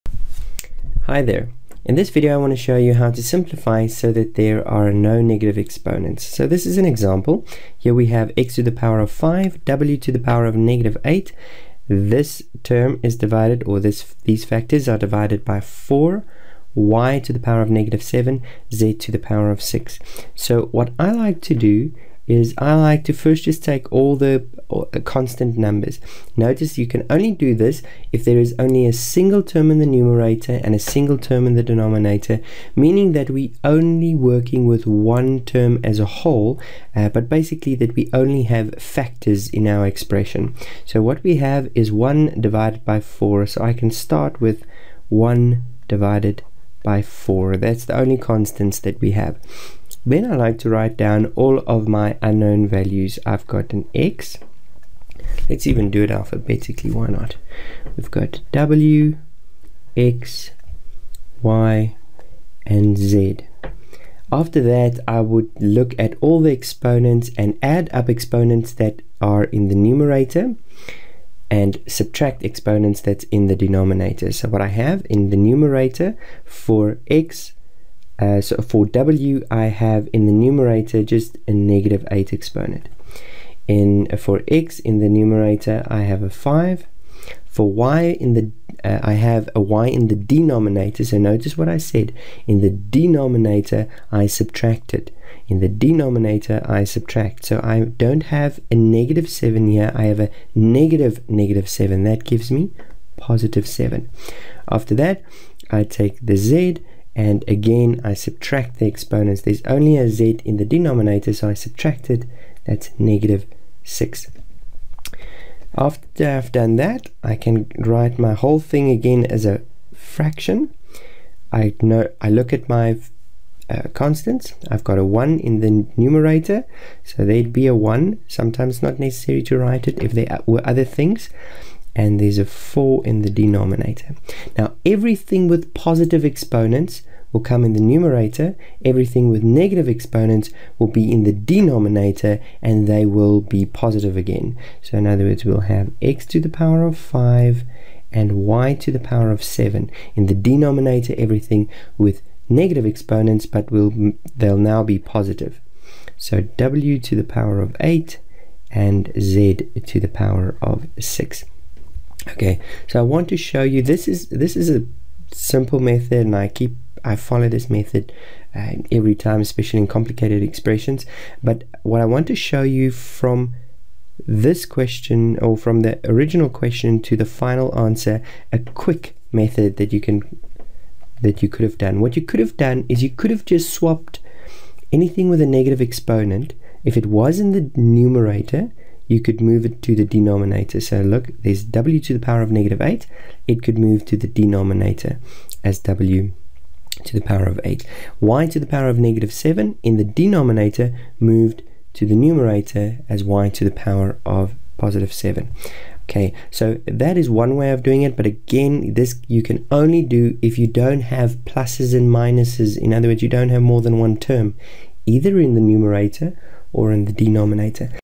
Mathematics|Algebra | Division| Negative Exponents | Example 1
Example of how to simplify fraction with negative exponents